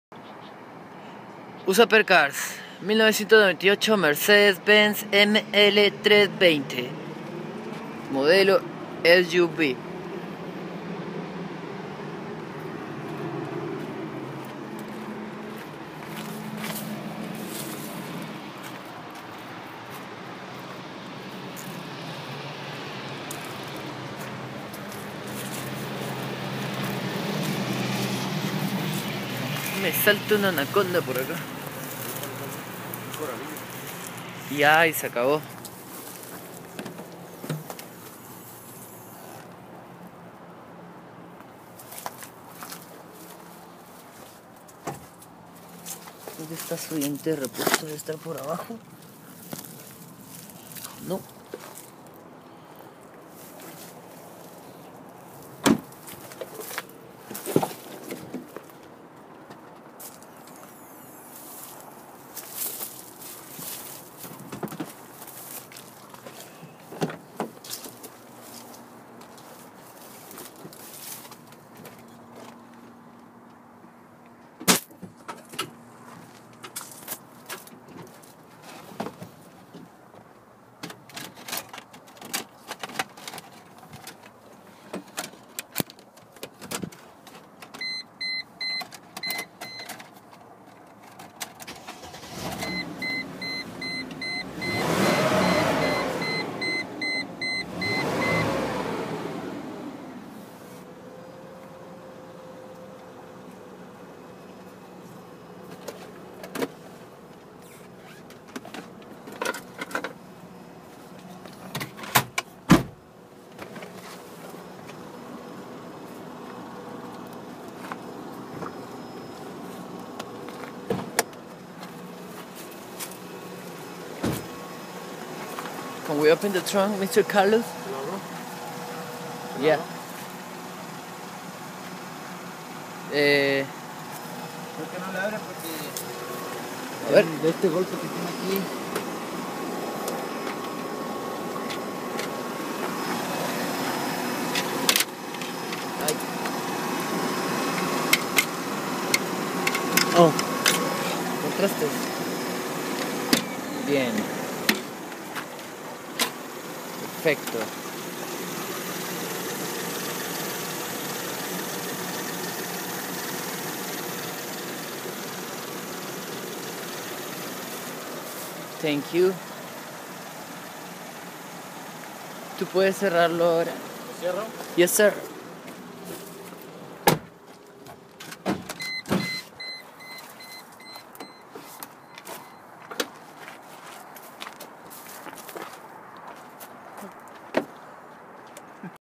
Importando Autos en Arica - Inspección mercedes benz
Nos contacto un importador de Arica pidiendo este espectacular Mercedes Benz y vemos con envidia sana que los Chilenos no tienen restricciones para importar autos, lo único que les importa es que el auto este apto para circular aprobado por una revision técnica super estricta sello verde

Saludos .